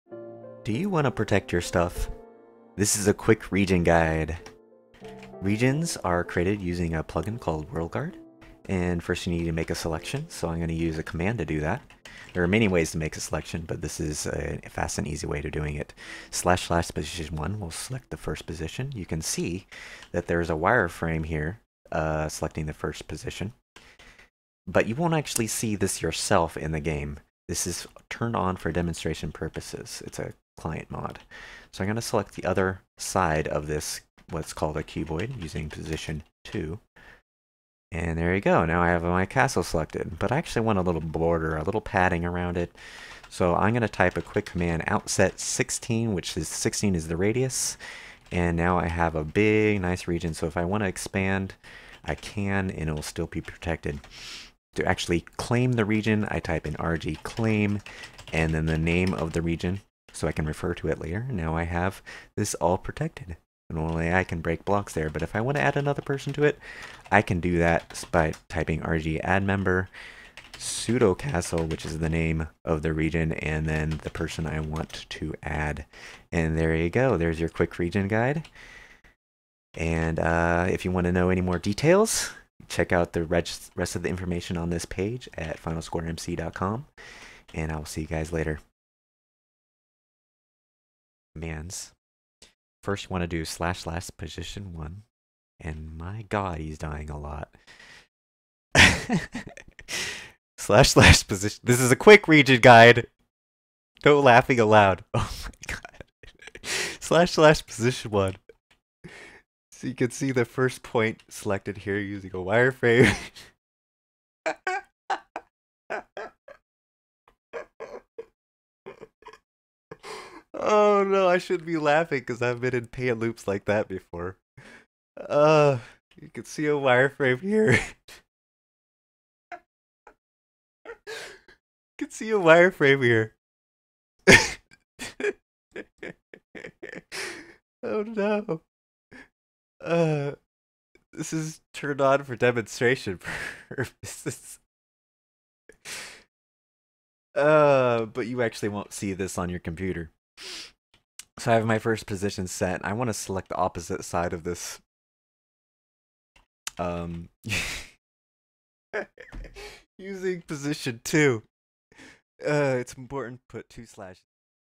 FSMC - Quick Region Guide
Demonstrating a quick way to create a region. More details on how to manage regions on the Final Score server can be found at the finalscoremc.com website.